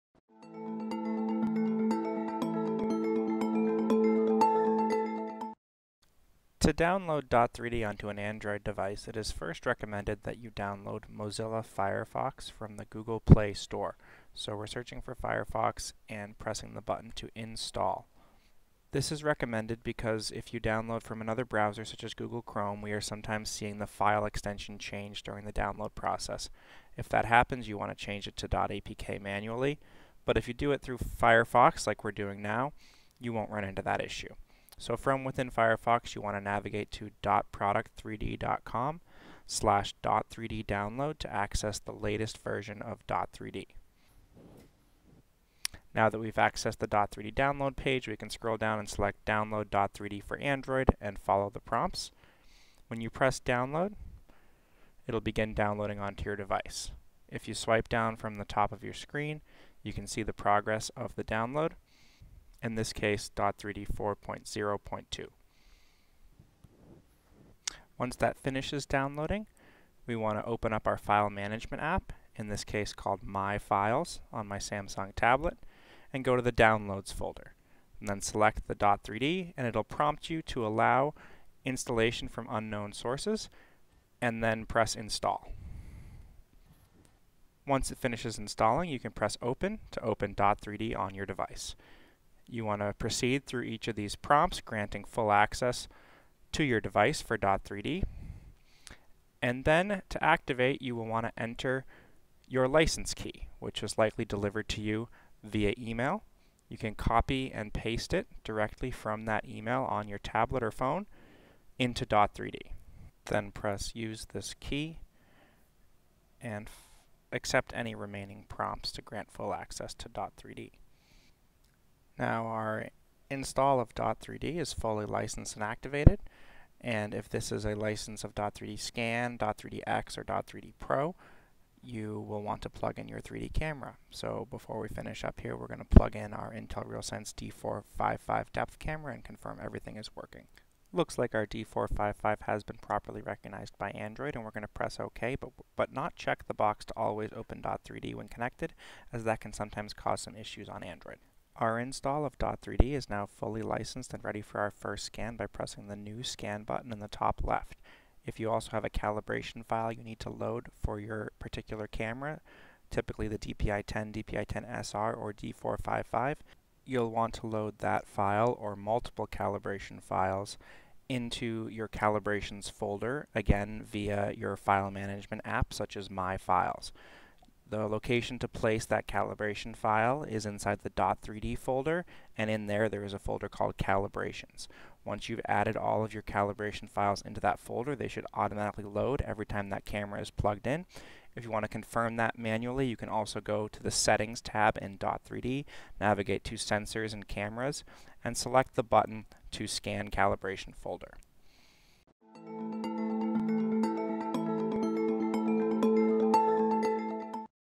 Tutorial: How to Download, Install, and Activate Dot3D on Android
This brief tutorial video shows how to download the latest release of Dot3D scanning software onto an Android tablet or phone install onto the device, and activate with a Dot3D license key. This procedure applies to all license versions of Dot3D: VIEW, EDIT, SCAN, X, and PRO.